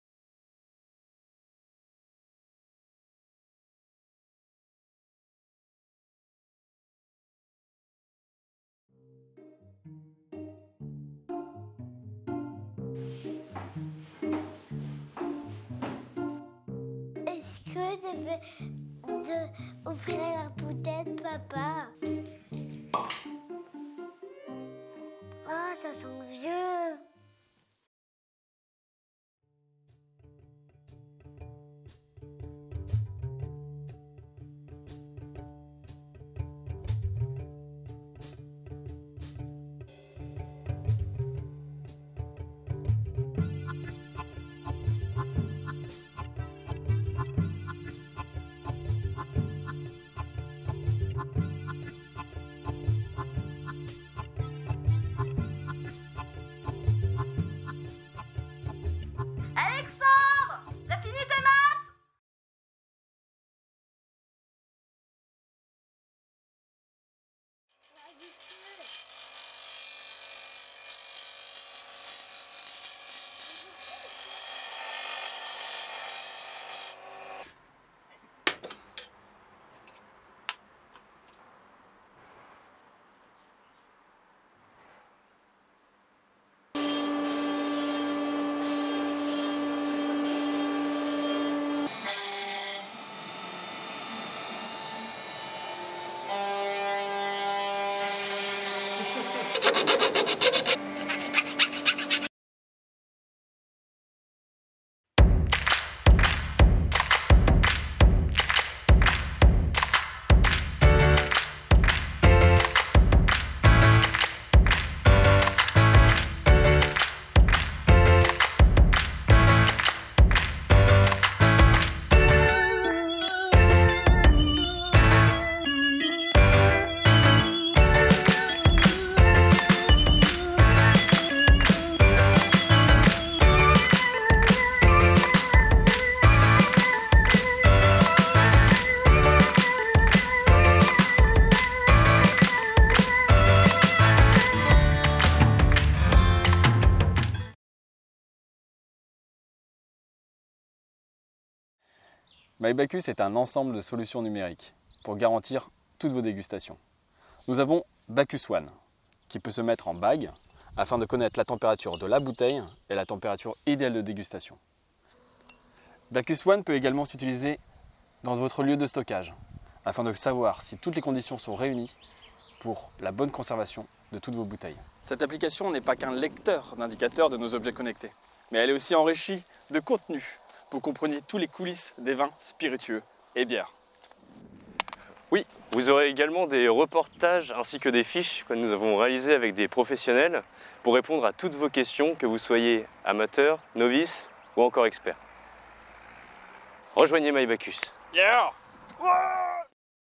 My Bacchus - D'une idée à Bacchus One - Teaser Historique
Comment est né My Bachus?
Une petite vidéo avec le fils du fondateur, ses amis et le talent de Johann Ménier, qui nous plonge dans l'histoire de notre startUp. Regardez ce court métrage, suivez nous et soutenez nous sur